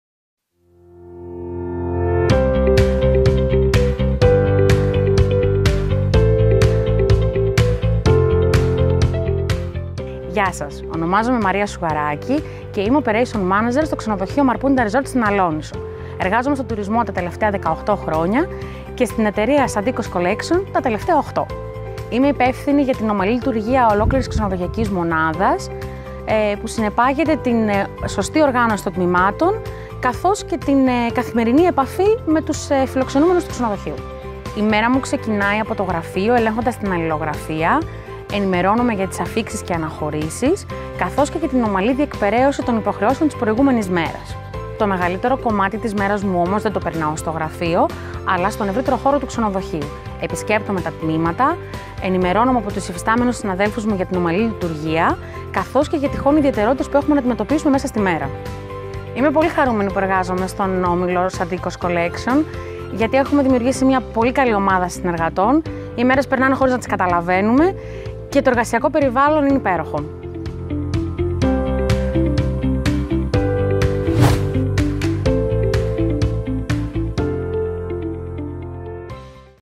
Maria Sougaraki, Operation Manager at Santikos Collection Marpunta Resort
We Tour We Listen @ Alonissos

Workathlon's initiative to Tour all over Greece and Listen what Hotel Employees have to say about their job in the most luxurious Hotels of the country. Come on board and meet them!
|Videoshooting - Montage: Sokratis Patsos|